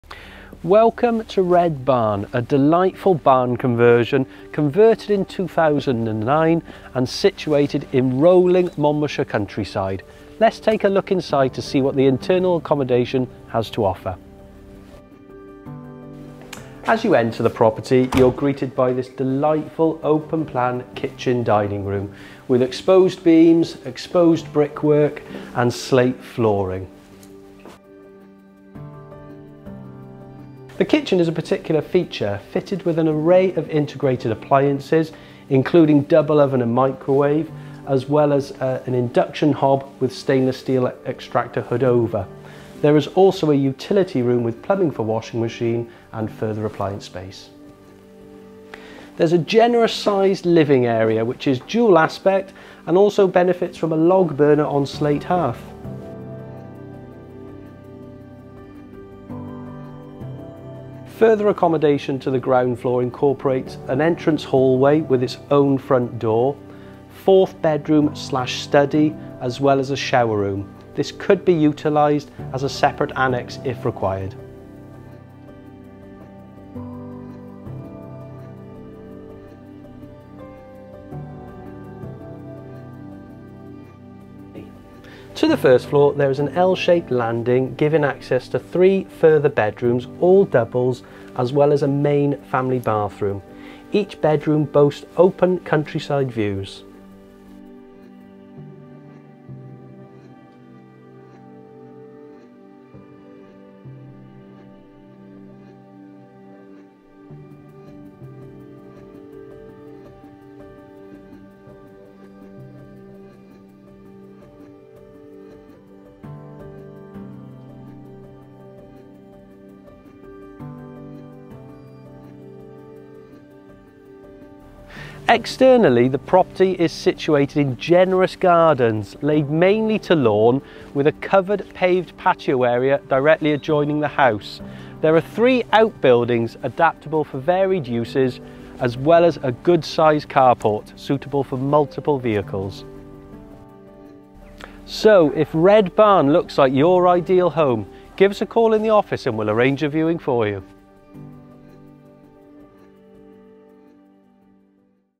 Red Barn | Archer & Co
A beautiful example of a barn conversion set within the Monmouthshire countryside and enjoying great road links to major towns. Large gardens and several outbuildings offer fantastic potential. Take the guided tour with award winning Estate Agents Archer & Co and Ben Bontes-Cutter.